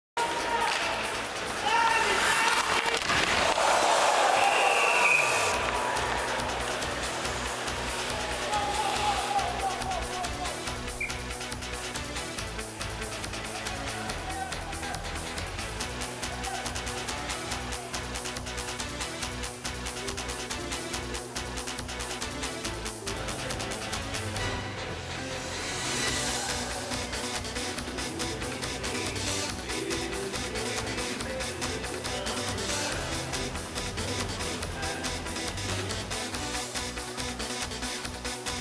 Dauphin hits Baldaev (QMJHL - Russia) Subway Super Series 2013. Game 2.
Subway Super Series 2013. Game 2. Sherbrooke, QC, November 20, 2013
Team QMJHL  4 : 3 Team  Russia
Laurent Dauphin hits Viktor Baldaev